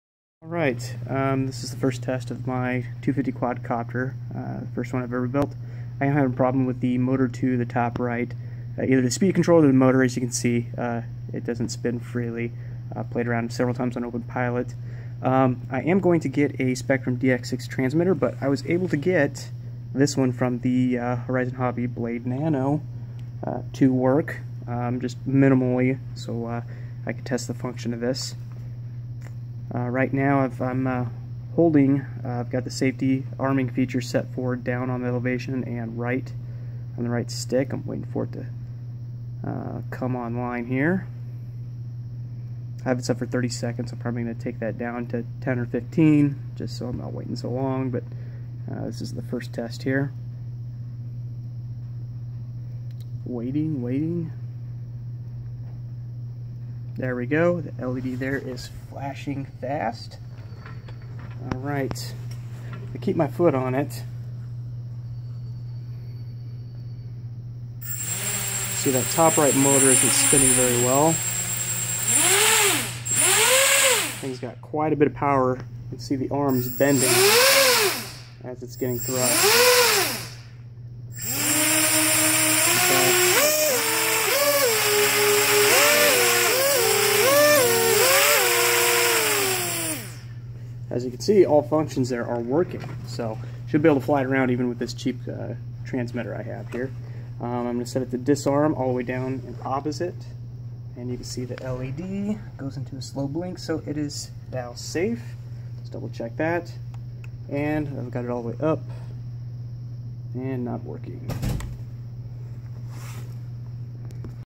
250 Quadcopter - 3D Printed Frame, CC3D Controller, AR610 Reciever, Blade MLP4DSM Transmitter
A big thank you to the friendly staff of Hobb-E-Mart at  2875 N Dirksen Pkwy, Springfield, IL. I went in not knowing much but successfully made my first build without any huge snags with their friendly service!

First test of my home build 250 quadcopter after programming in OpenPilot. I have an issue with either the motor and/or the ESC for the front right which I need to debug. If anyone knows the issue, please let me know!

The frame is 3D printed by a local person so I wanted to try it out for my first setup. The controller is of course the OpenPilot CC3D Evo. The motors are EMAX MT 1804-KV2480, running off EMAX Simon Series 12A ESCs. The receiver is a Spektrum AR610 that a friend gave me.  I used a power distribution board with switches for the LED lights to make cable management and overall setup a little easier.  I added green LED strips to the front, and white LED strips to the back, wired in parallel. If you wire in series, even with a 2x2, there will not be enough current to power them.  Best to wire each directly to the power distribution board.

Surprisingly the cheap transmitter for the Horizon Hobby Blade Nano QX I used to learn the flight mechanics of quadcopters works with this setup! I will be using a Spektrum DX6 transmitter as soon as I have the extra $200 to get one...